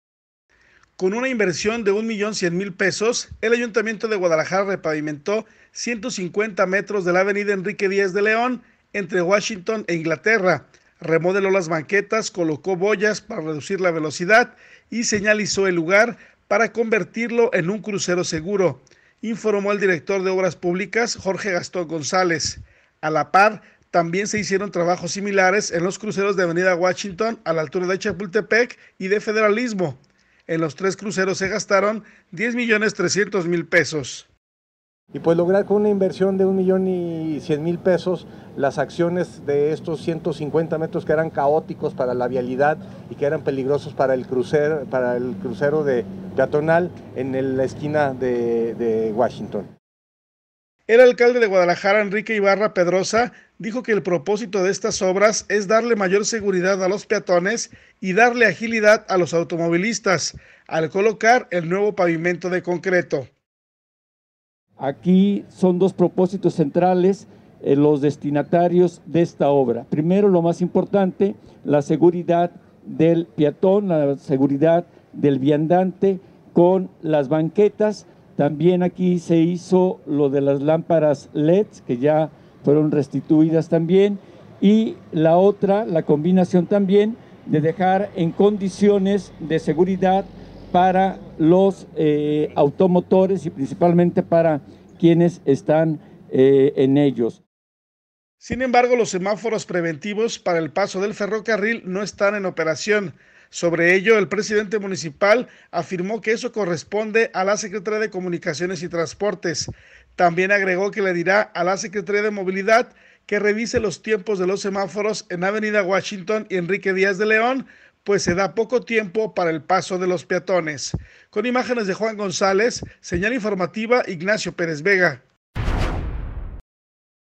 Convierten en “crucero seguro” la avenida Enrique Díaz de León e Inglaterra
Convierten en “crucero seguro” la avenida Enrique Díaz de León e
Inglaterra.